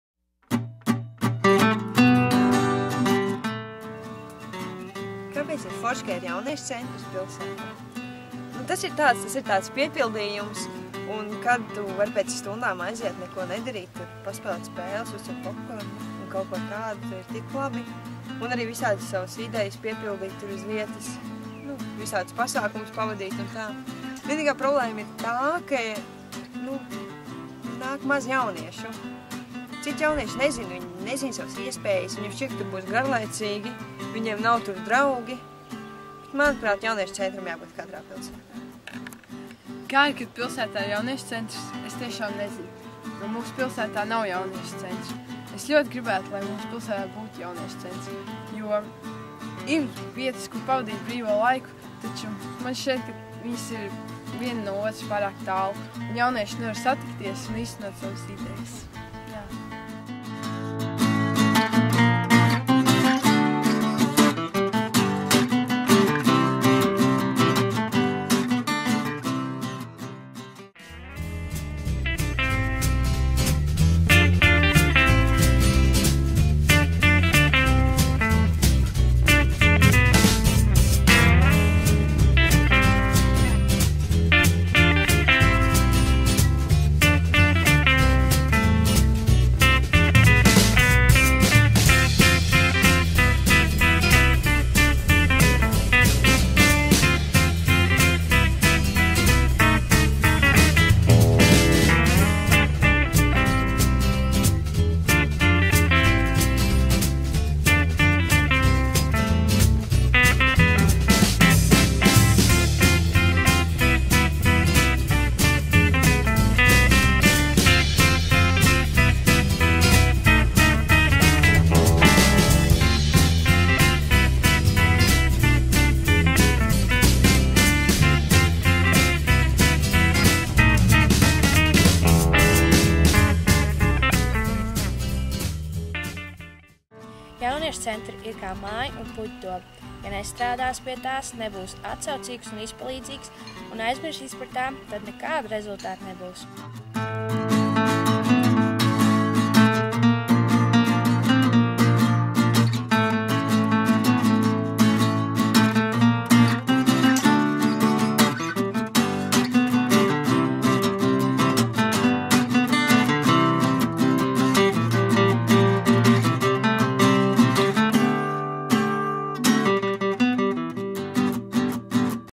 Jauniešu centrs kā māja un dobe
Biedrības "NEXT" jauniešu līdzdalības semināra "Man ir ko teikt!" dalībnieku veidoti video par jauniešiem aktuāliem jautājumiem.

The final presentations by the participants attending NEXT youth organisation's seminar "Man ir ko teikt!" (I have something to say!) put together a series of videos which addressed major issues they found needed addressing in Latvia and the world.